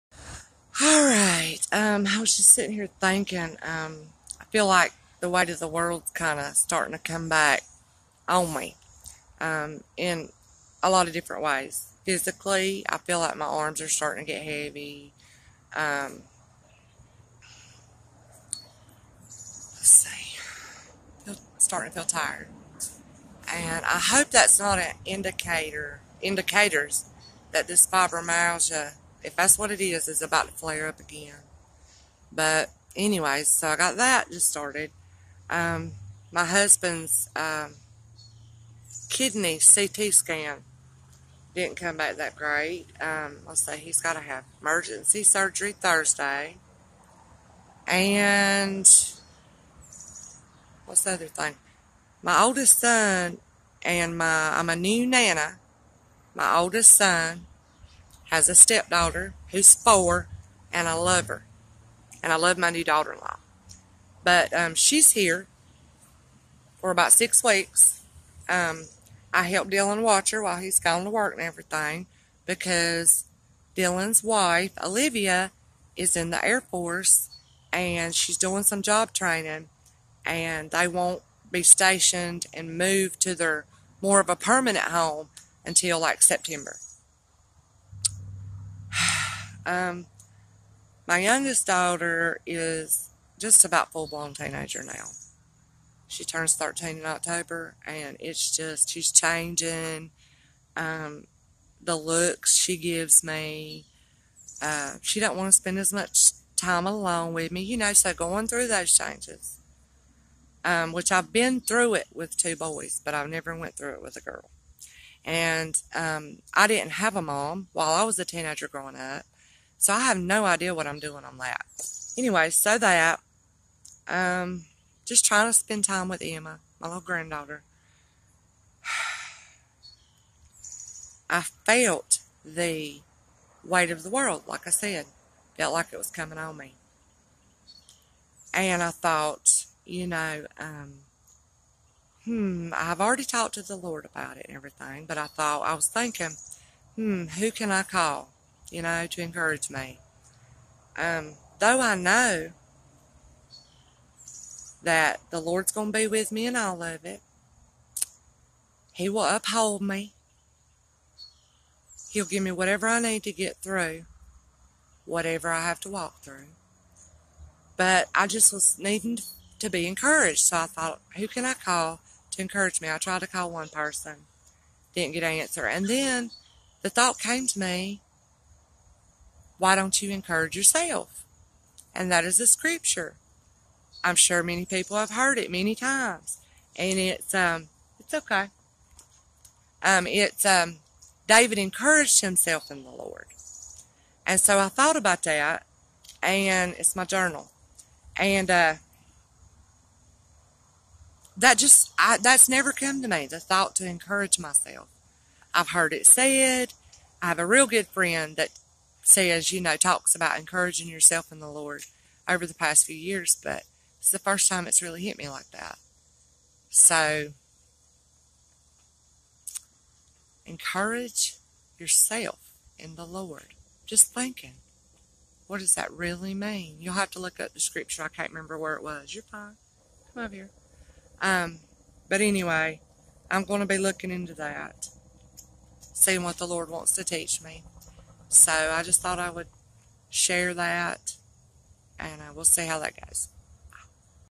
Encourage yourself.
I needed some encouragement from a friend. No one was available, I heard, "why don't you encourage yourself". Instantly, I remembered the scripture about David encouraged himself in the Lord. 072418